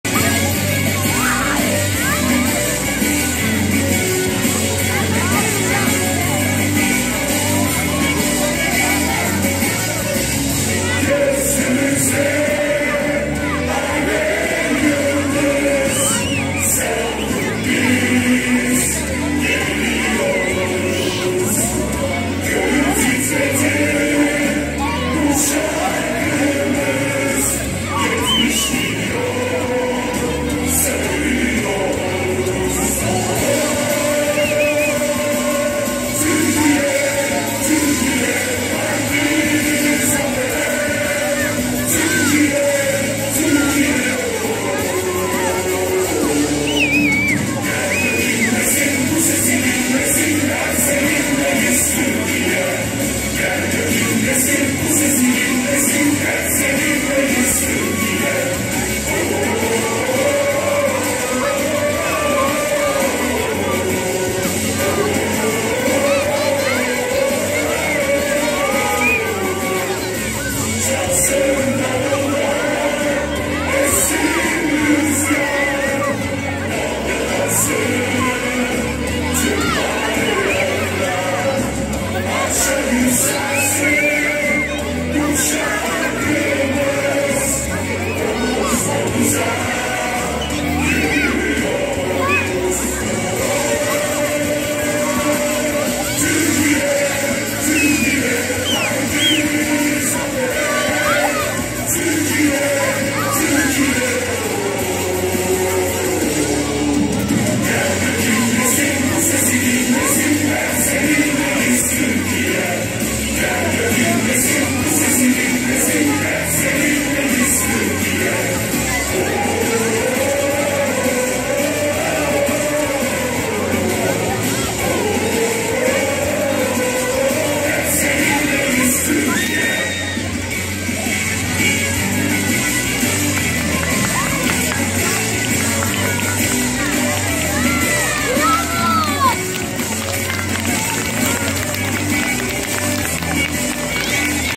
23 Nisan Hep Seninleyiz Türkiye Gaziantep Fatih Sultan Mehmet İlkokulu 3-I Sınıfı
En güzel 23 Nisan gösterisi- Muhteşem ve kolay